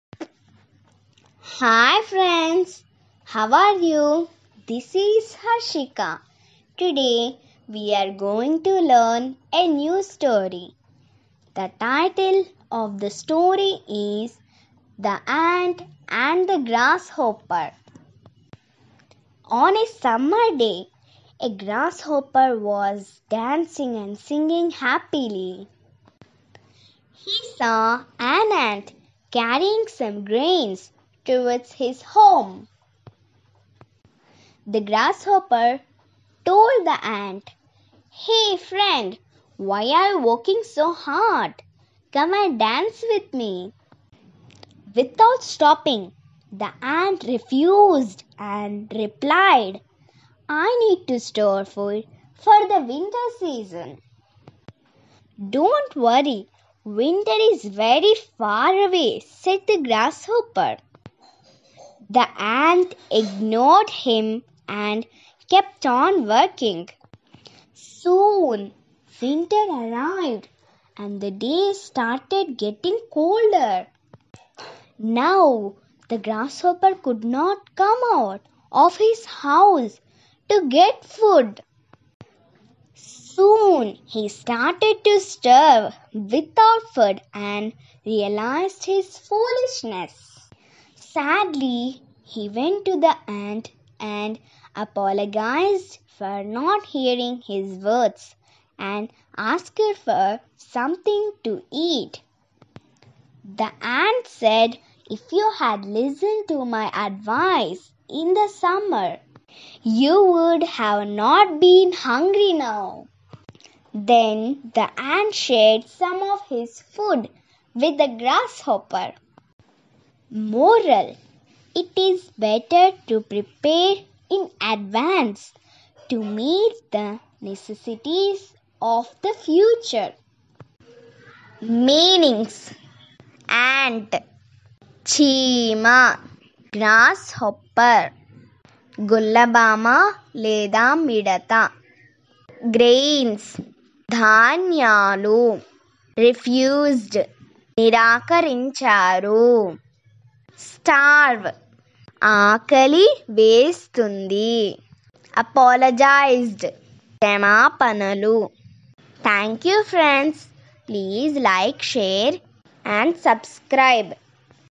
THE ANT AND THE GRASSHOPPER
LEARN ENGLISH THROUGH STORIES WITH ENGLISH TO TELUGU MEANINGS